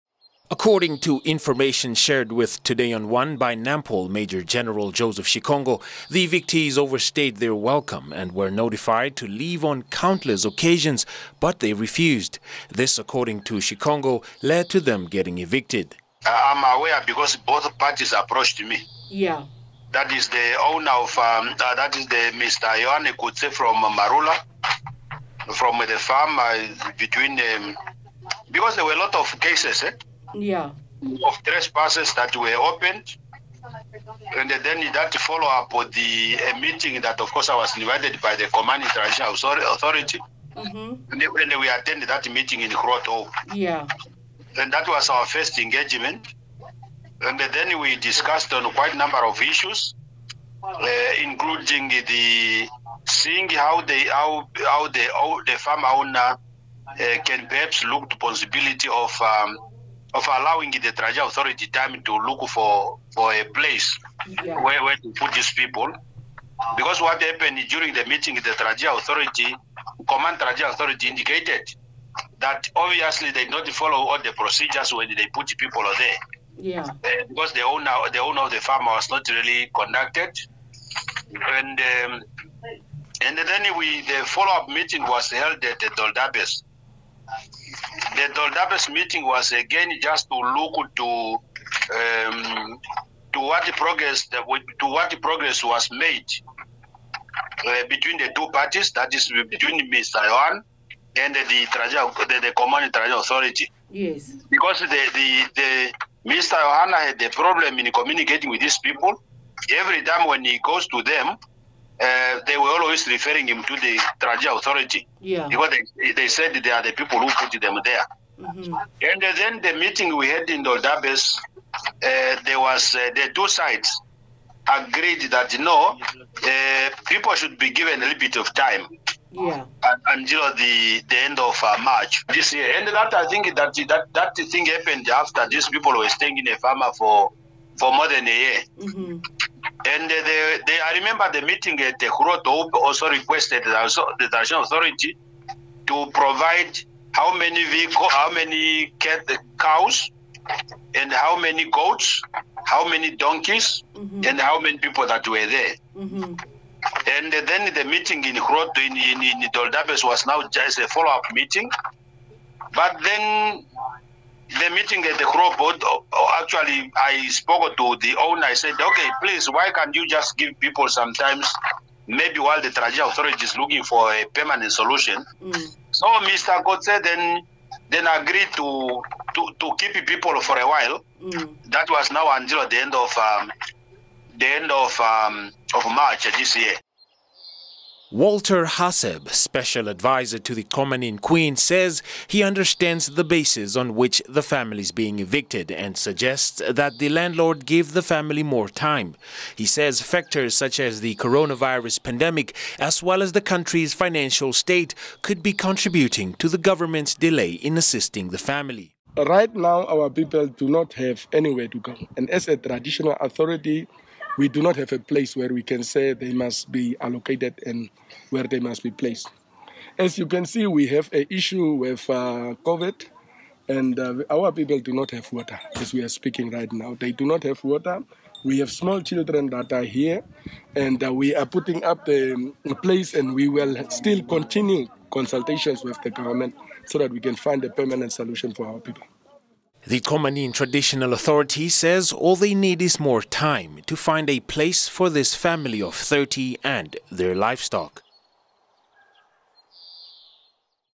Farmers overstayed their welcome
Further investigation into the farm eviction near Dordabis shows that the General Manager of the farm, after a number of consultations with the Traditional Authority and Namibian Police, followed valid legal procedures
The farmers overstayed their welcome after an arrangement allowing them more than 12 months to move, which expired in March 2021.